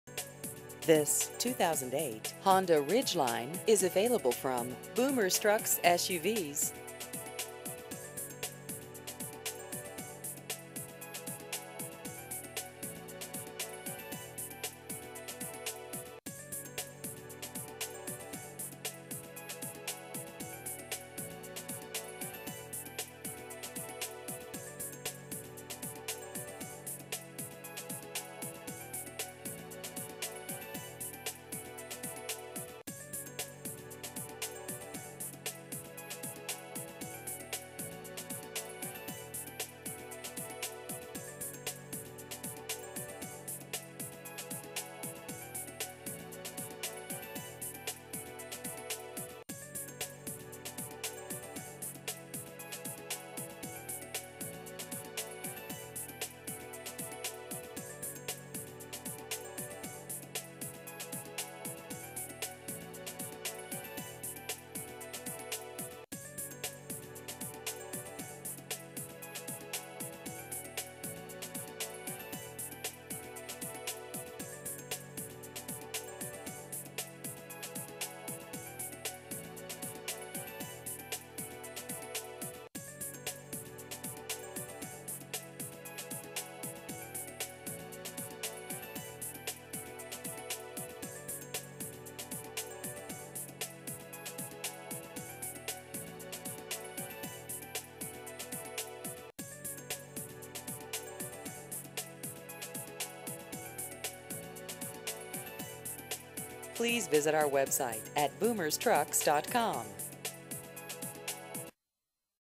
2008 Honda Ridgeline Used Cars Orlando Longwood Sanford FL
This 2008 Honda Ridgeline is available from Boomers Truck's SUV's, LLC.